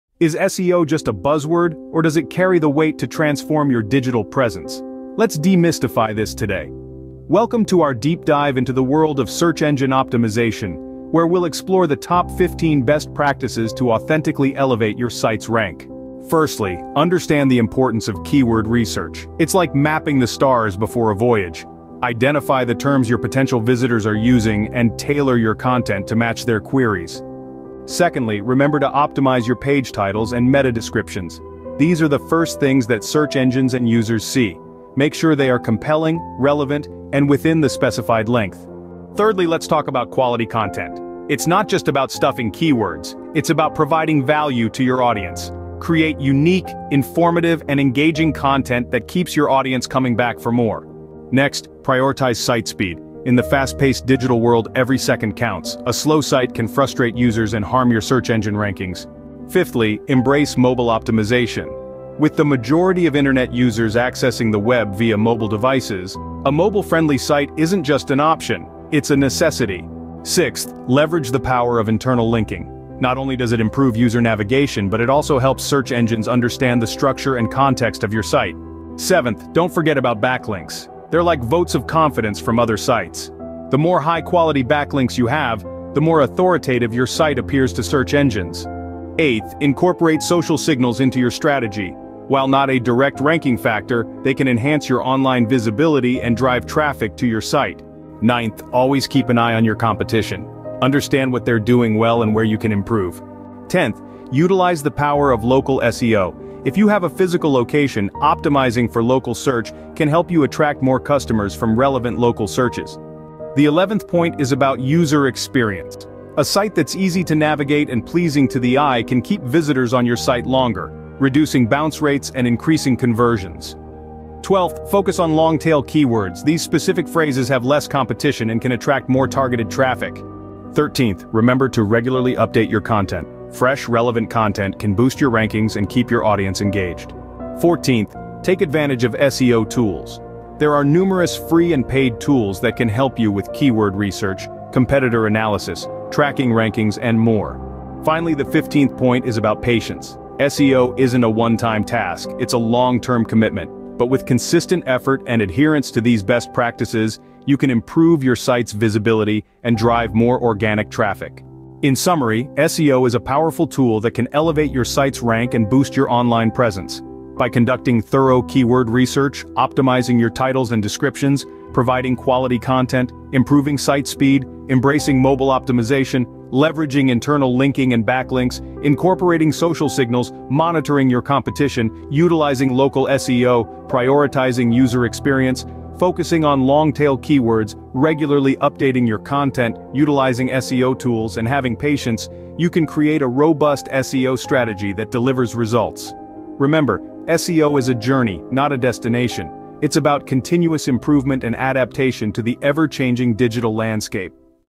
Top 15 Best SEO Practices - Rank #1 on Google
Dive into the world of SEO with our latest video, "15 Best Practices for SEO Mastery." Unlock the secrets to boosting your website's visibility on search engines and learn the most effective strategies to enhance your online presence. From keyword research to mobile optimization, and content creation to backlink building, we've compiled the top tactics that every digital marketer and website owner needs to know. Join us as we explore actionable tips and insider insights to propel your site to the top of search rankings. Engage with us in the comments below to share your SEO experiences and questions.